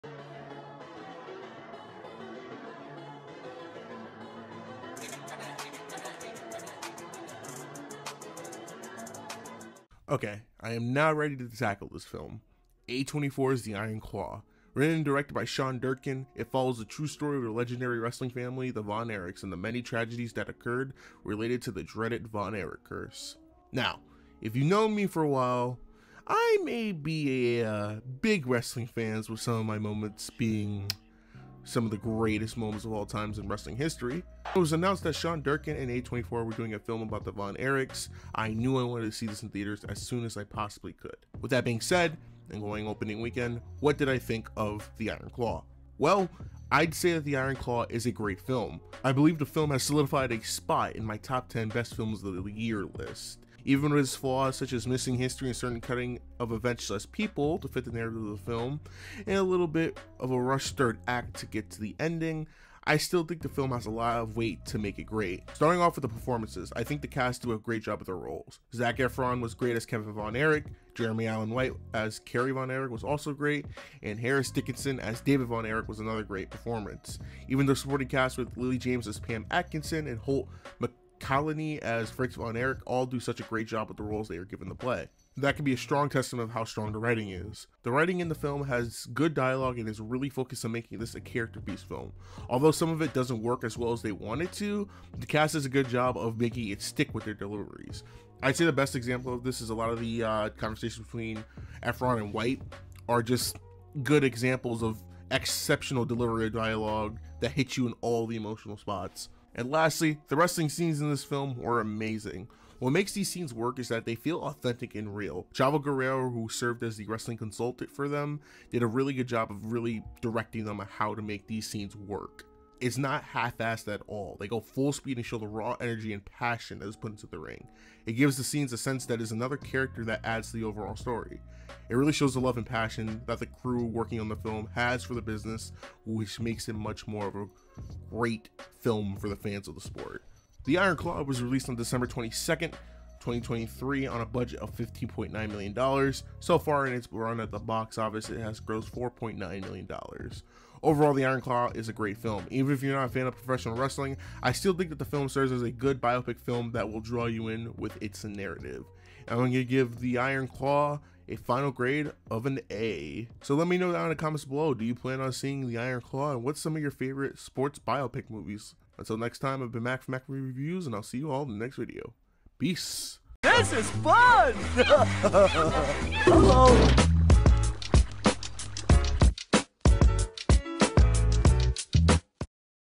The Iron Claw (2023) Review: A24 Ending 2023 Strong
One of the films I was excited for this year done by one of my favorite studios. Let's do this. Here's my review of 2023's The Iron Claw directed by Sean Durbin and starring Zac Efron and Jeremy Allen White.